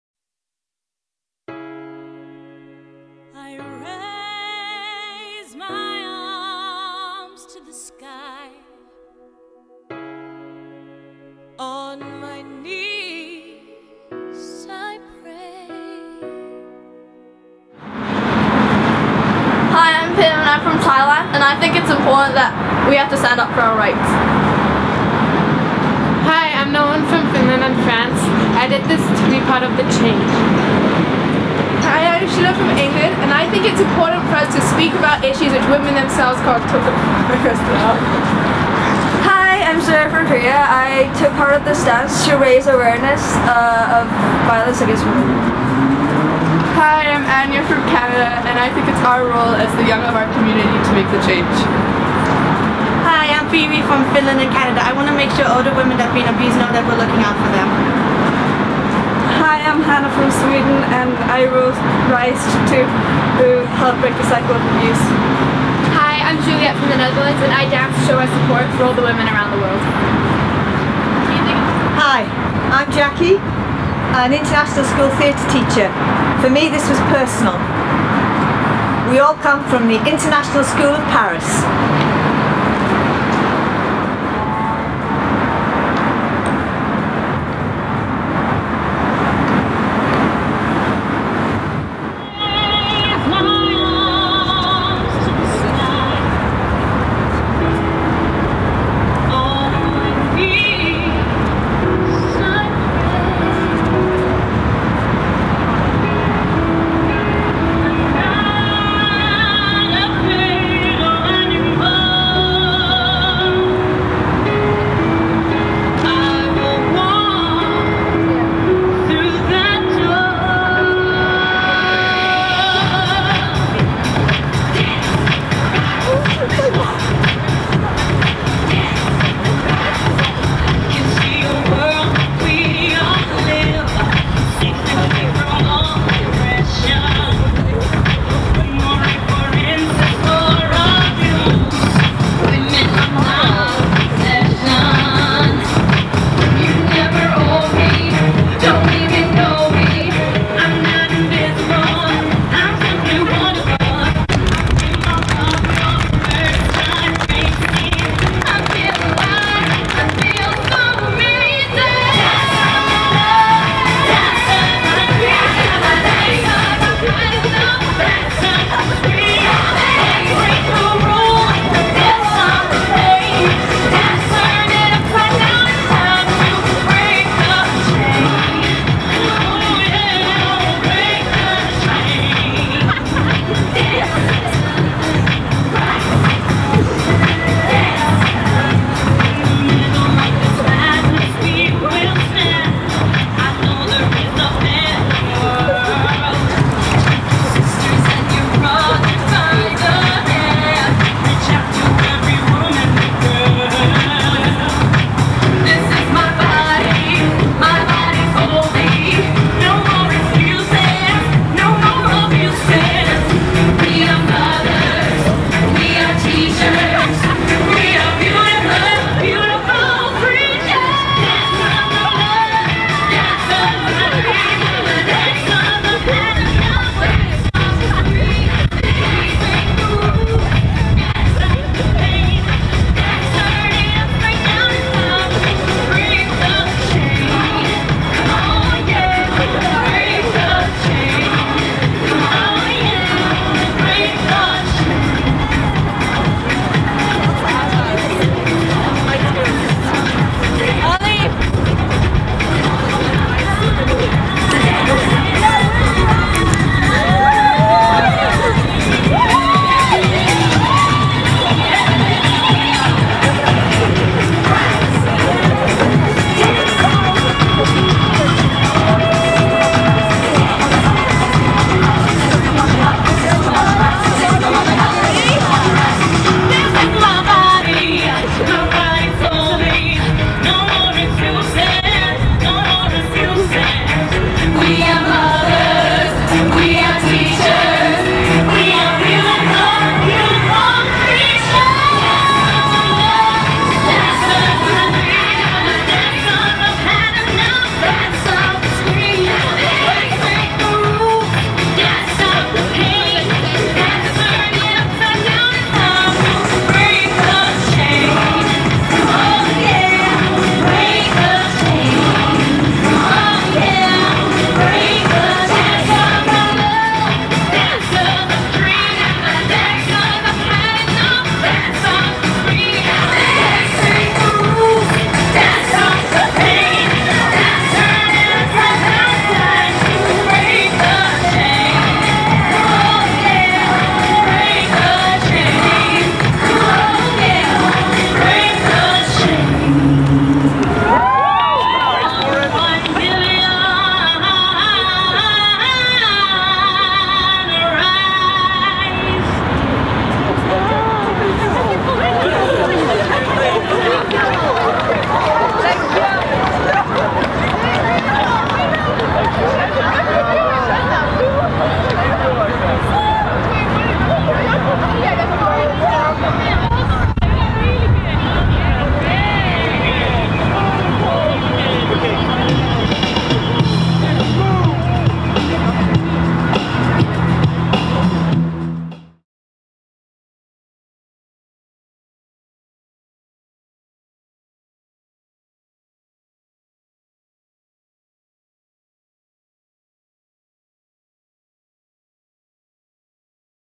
International School of Paris One Billion
Students rising and dancing in Trocadero Park for 1 Billion Rising...